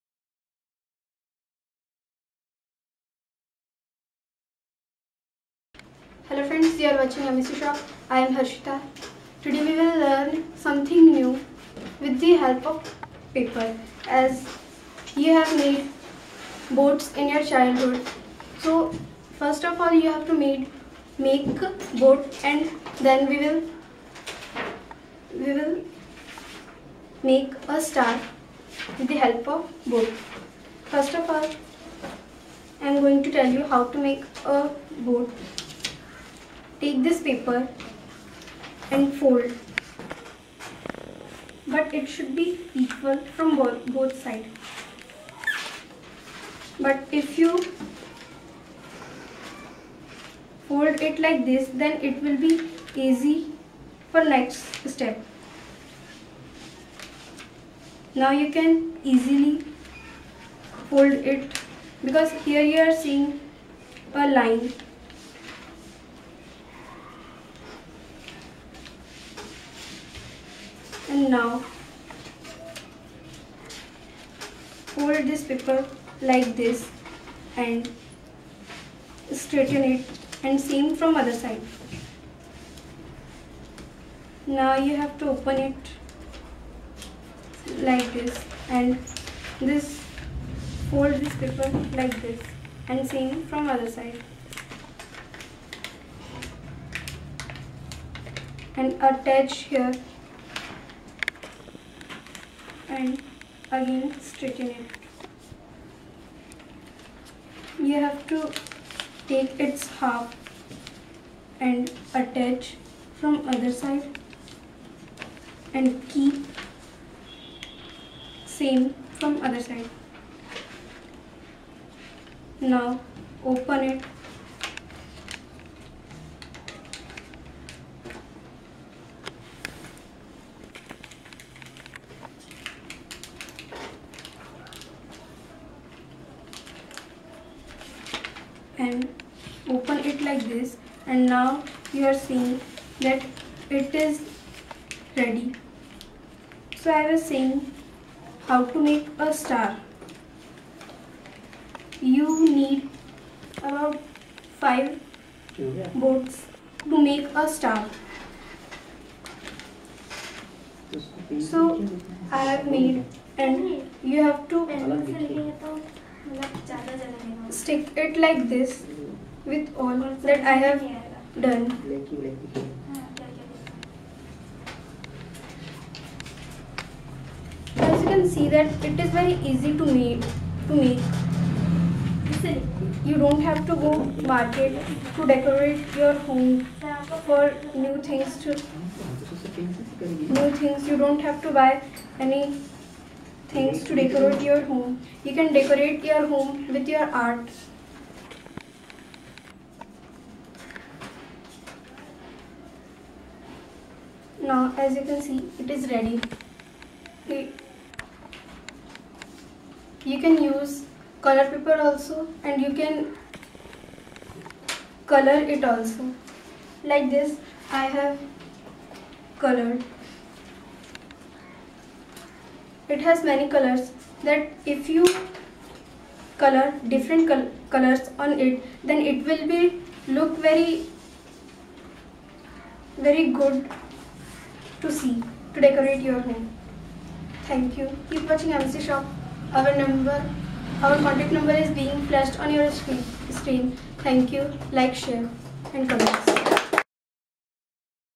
SalesGirl Act@MECSHOP
Harshita inculcating paper craft art ,,how to make Decorative Star